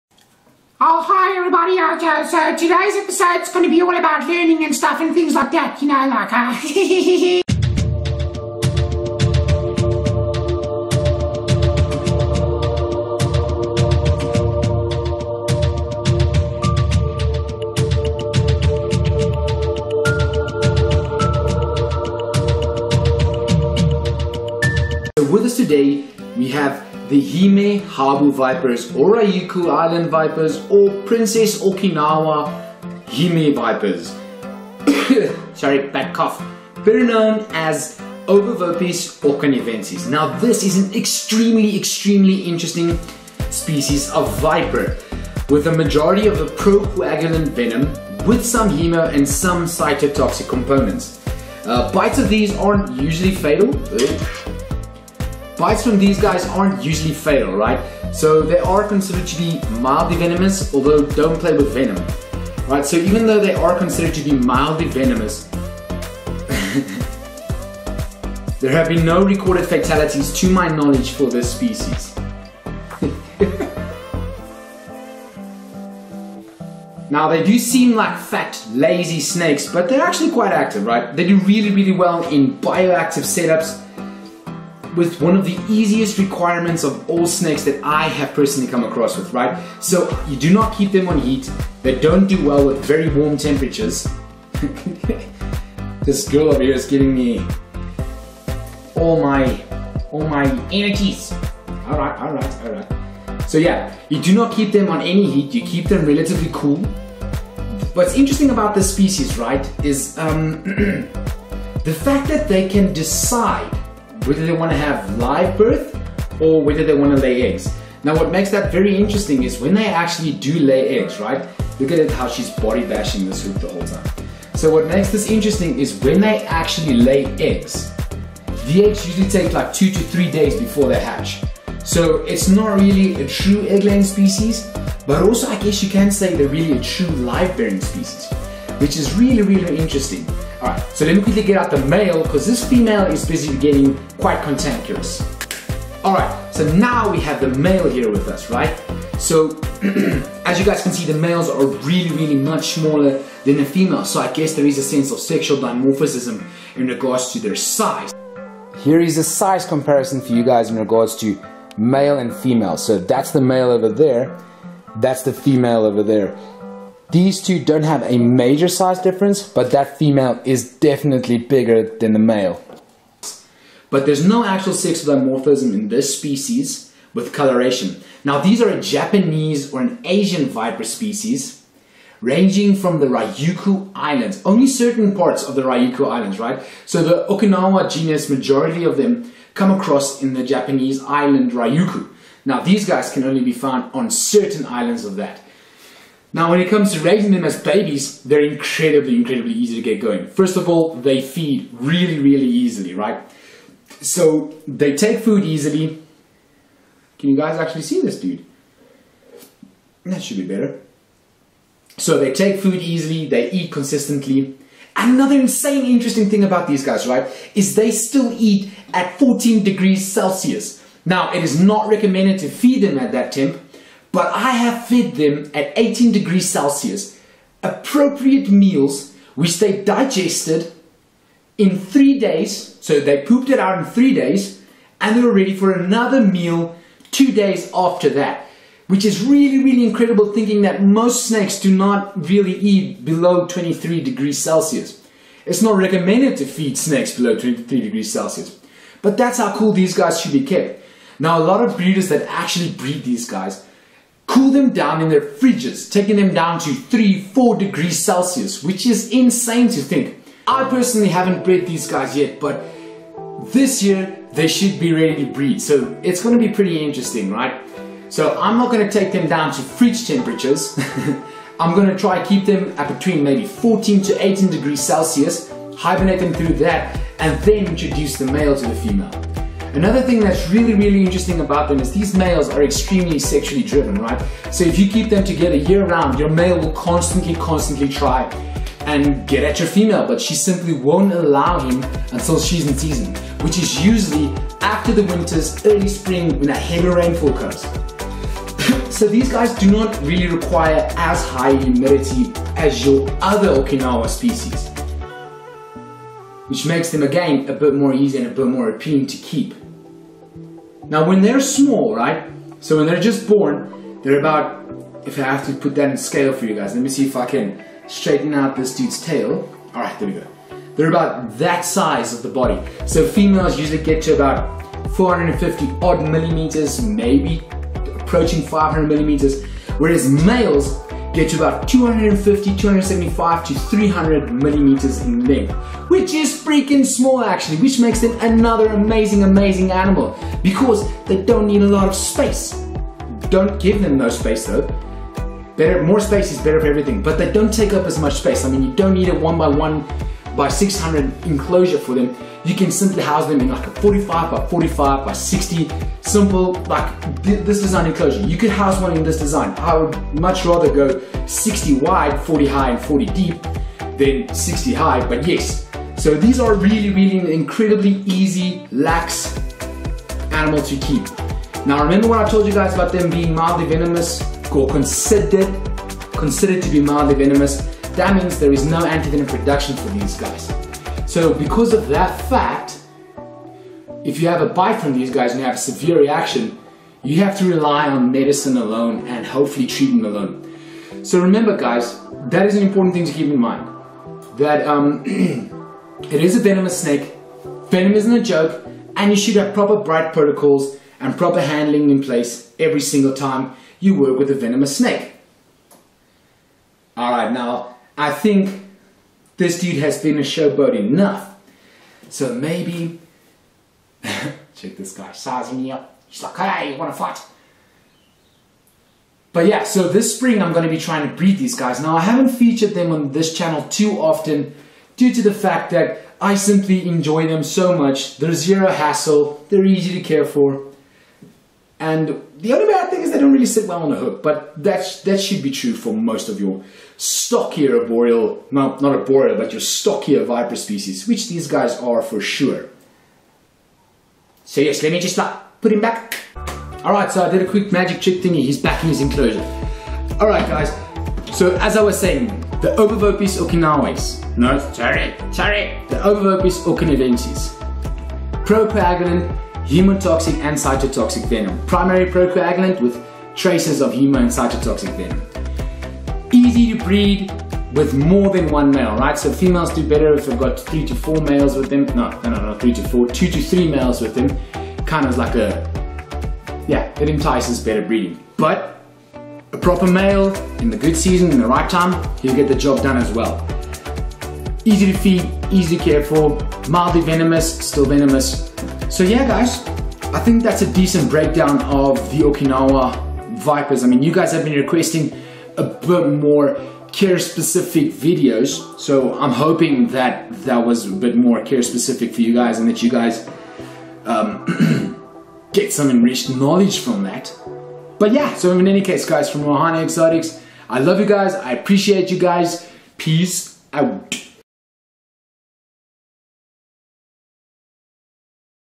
HOW TO CARE FOR OKINAWA PRINCESS HABU VIPERS*they are royalty*
the hime habus go by many different names names like hime habus,princess habu vipers,ryuku island vipers and many more which is a good example of why latin names are so important!because they just have the one O.okinavensis

these are without a doubt my favorite species to keep and it says allot about them concidering the fact that i am an huge arboreal viper fan!

hope you guys enjoy the quick burst of information i share about them and that you guys learn a little something something